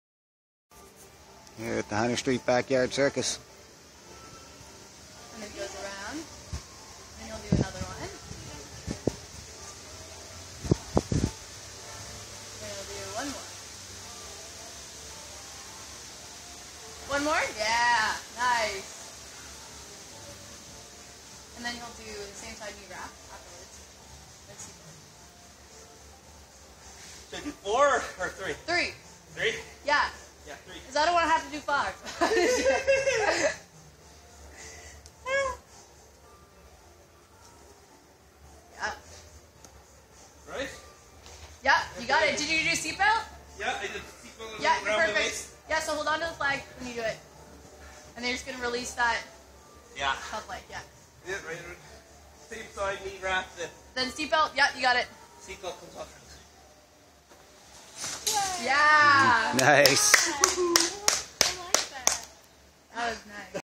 Hunter Street Backyard Circus - Aerial Silks - The Drop
The drop trick performed on the aerial silks at the Hunter Street Backyard Circus.

The silks are hanging from a cable strung between two huge maple trees. Aerial circus rope can also be hung from the cable and the cable itself can be used as a very short zipline.

Be sure to check out some of the aerial performances I have on YouTube using these silks.